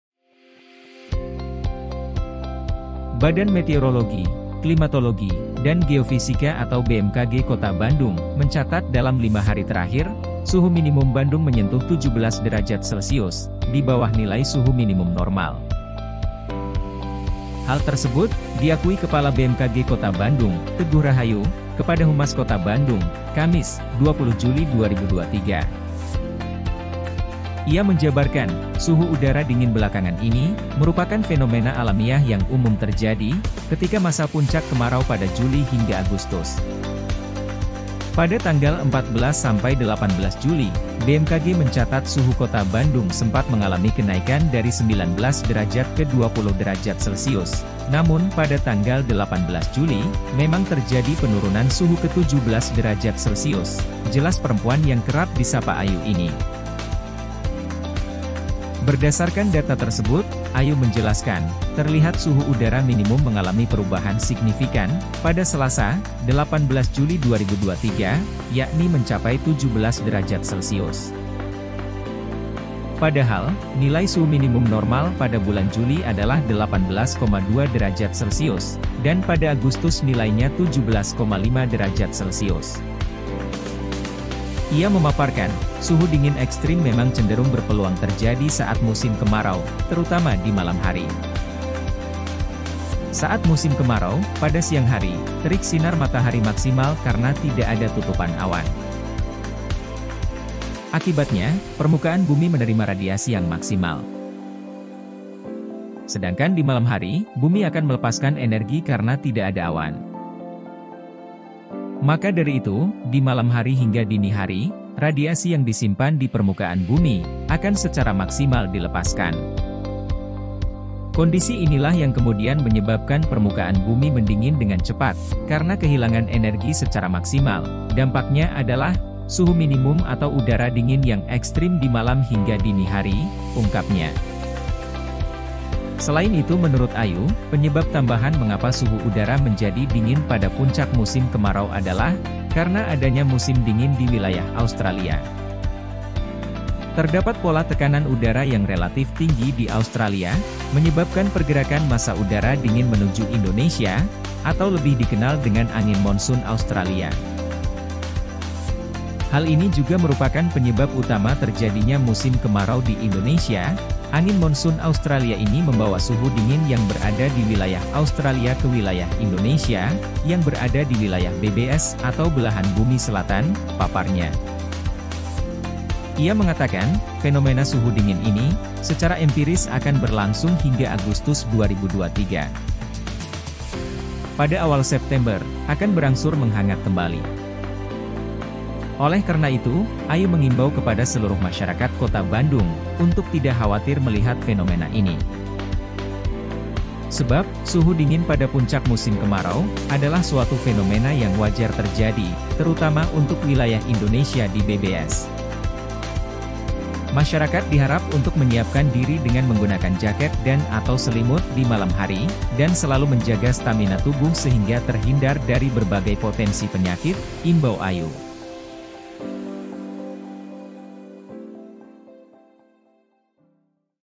Suhu Udara di Bandung Terasa Lebih Dingin? Ini Penjelasan BMKG
Jubir TV, Bandung - Badan Meteorologi, Klimatologi, dan Geofisika (BMKG) Kota Bandung mencatat, dalam 5 hari terakhir (14-18 Juli 2023) suhu minimum Bandung menyentuh 17 derajat celsius, di bawah nilai suhu minimum normal.

Hal tersebut diakui Kepala BMKG Kota Bandung, Teguh Rahayu kepada Humas Kota Bandung, Kamis (20/7/2023).

Ia menjabarkan, suhu udara dingin belakangan merupakan fenomena alamiah yang umum terjadi ketika masa puncak kemarau pada Juli-Agustus.

"Pada tanggal 14-18 Juli, BMKG mencatat suhu Kota Bandung sempat mengalami kenaikan dari 19° ke 20°C. Namun pada tanggal 18 Juli memang terjadi penurunan suhu ke 17°C," jelas perempuan yang kerap disapa Ayu ini.

Berdasarkan data tersebut, Ayu menjelaskan, terlihat suhu udara minimum mengalami perubahan signifikan pada Selasa, 18 Juli 2023 yakni mencapai 17°C.

Padahal nilai suhu minimum normal pada bulan Juli adalah 18,2°C, dan pada Agustus nilainya 17,5°C.

Ia memaparkan, suhu dingin ekstrem memang cenderung berpeluang terjadi saat musim kemarau, terutama di malam hari.

Saat musim kemarau, pada siang hari, terik sinar matahari maksimal karena tidak ada tutupan awan.

Akibatnya permukaan bumi menerima radiasi yang maksimal.

Sedangkan di malam hari, bumi akan melepaskan energi karena tidak ada awan.

Maka dari itu, di malam hari hingga dini hari, radiasi yang disimpan di permukaan bumi akan secara maksimal dilepaskan.

"Kondisi inilah yang kemudian menyebabkan permukaan bumi mendingin dengan cepat karena kehilangan energi secara maksimal. Dampaknya adalah suhu minimum atau udara dingin yang ekstrem di malam hingga dini hari," ungkapnya.

Selain itu, menurut Ayu, penyebab tambahan mengapa suhu udara menjadi dingin pada puncak musim kemarau adalah karena adanya musim dingin di wilayah Australia.

Terdapat pola tekanan udara yang relatif tinggi di Australia menyebabkan pergerakan masa udara dingin menuju Indonesia atau lebih dikenal dengan Angin Monsun Australia.

"Hal ini juga merupakan penyebab utama terjadinya musim kemarau di Indonesia. Angin Monsun Australia ini membawa suhu dingin yang berada di wilayah Australia ke wilayah Indonesia yang berada di wilayah BBS (Belahan Bumi Selatan),” paparnya.

Ia mengatakan, fenomena suhu dingin ini secara empiris akan berlangsung hingga Agustus 2023.

Pada awal September akan berangsur menghangat kembali.

Oleh karena itu, Ayu mengimbau kepada seluruh masyarakat Kota Bandung untuk tidak khawatir melihat fenomena ini.

Sebab suhu dingin pada puncak musim kemarau adalah suatu fenomena yang wajar terjadi terutama untuk wilayah Indonesia di BBS.

"Masyarakat diharap untuk menyiapkan diri dengan menggunakan jaket dan atau selimut di malam hari dan selalu menjaga stamina tubuh sehingga terhindar dari berbagai potensi penyakit," imbau Ayu.